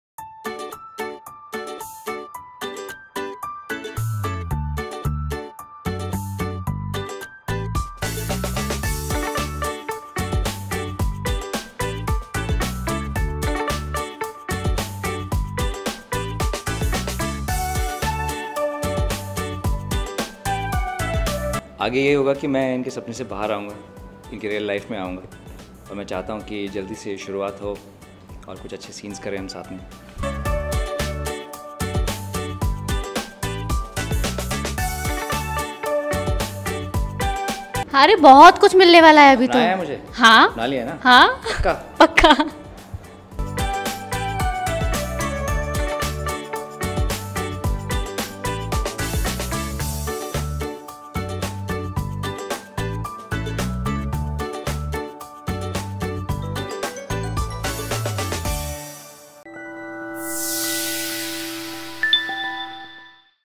Gudiya Humari Sabhi Pe Bhari: Gudiya gets married to Muddhu
In the upcoming episode of Gudiya Humari Sabhi Pe Bhari, audiences will get to see Gudiya (Sarika Bahroliya) and Muddhu's wedding. Yes, you read that right. Finally, Gudiya will get married to the boy of her dreams Muddhu (Sarrtaj Gill). The two will get married in the presence of their family and friends. Actor Sarrtaj who plays the role of Muddhu talks about the upcoming interesting scenes in the show. Watch their exclusive interview here -